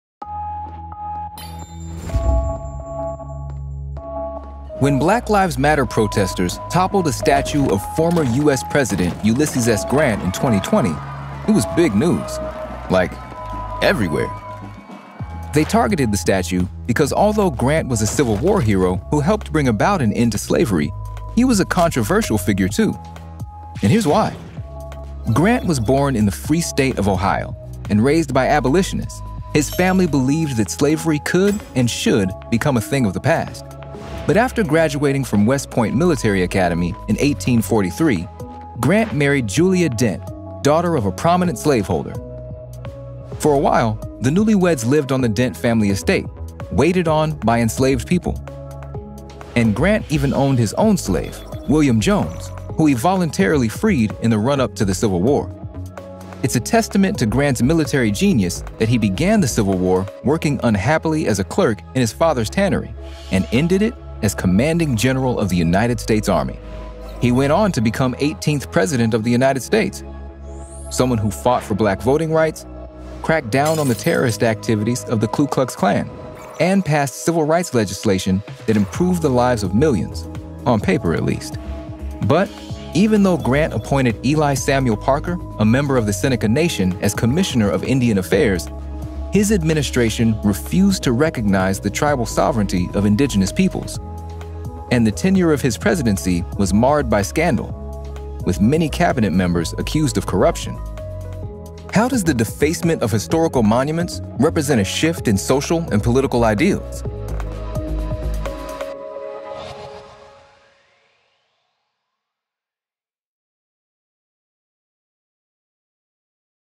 Ulysses S Grant: Profile of a Leader
When Black Lives Matter protesters toppled a statue of former US President Ulysses S Grant in 2020, it was big news! They targeted the statue because although Grant was a Civil War hero who helped bring about an end to slavery, he was a controversial figure too – and here’s why.